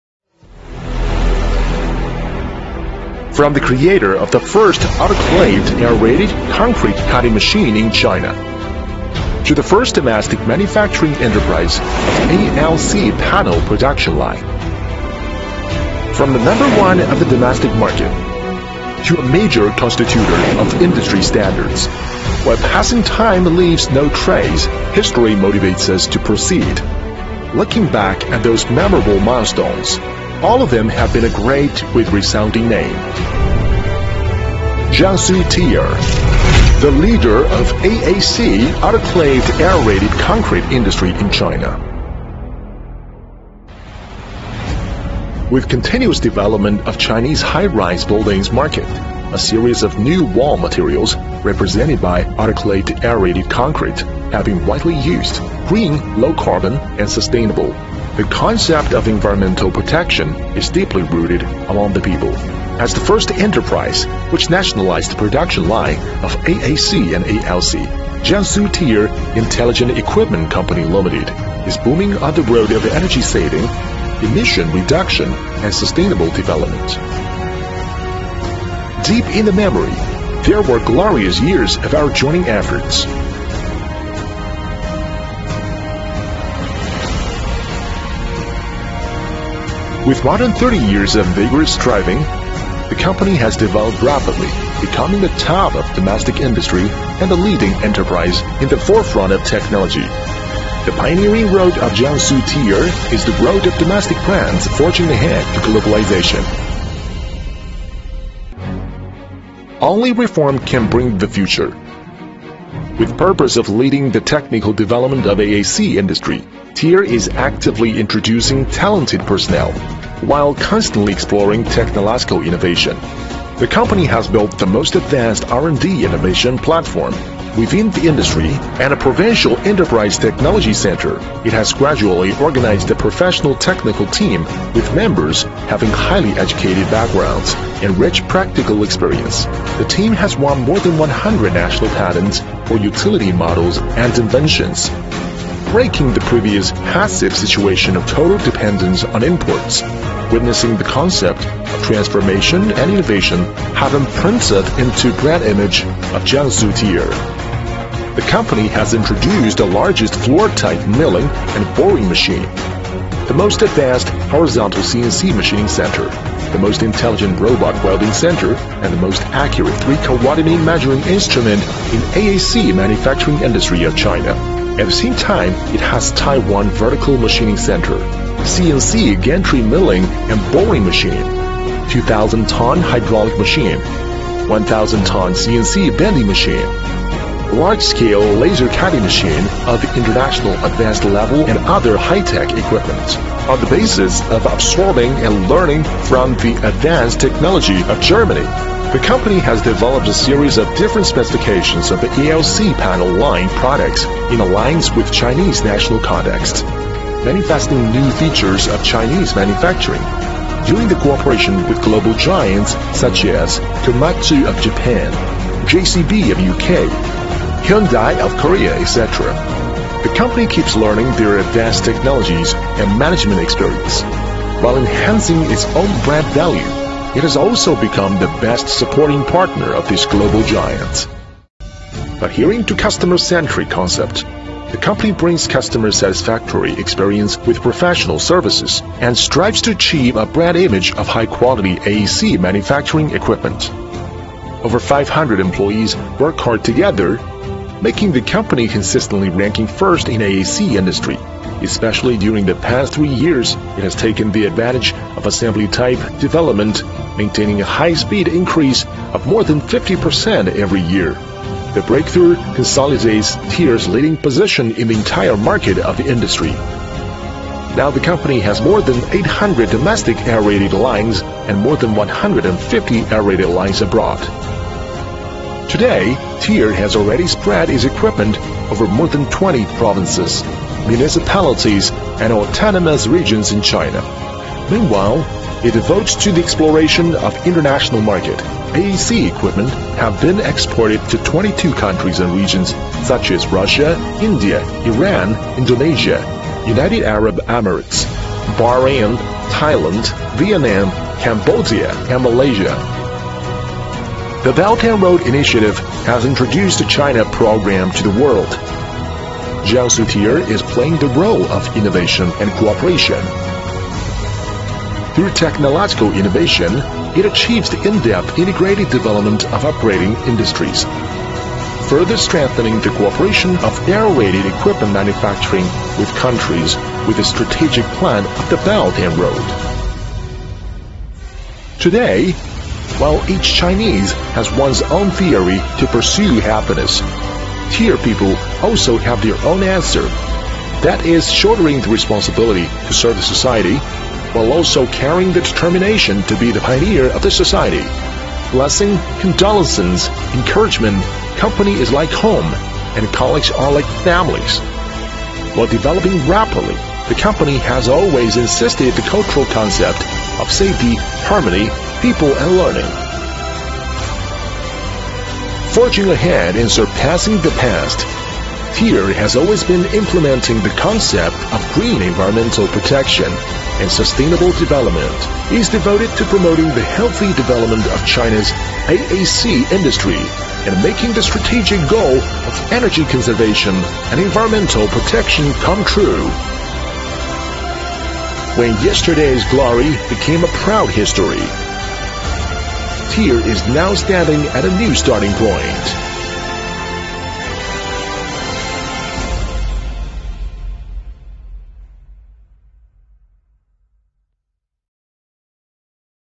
Teeyer intelligent equipment, leading position in aerated autoclaved concrete industry AAC ALC panel
General introduction of Teeyer company in Changzhou city China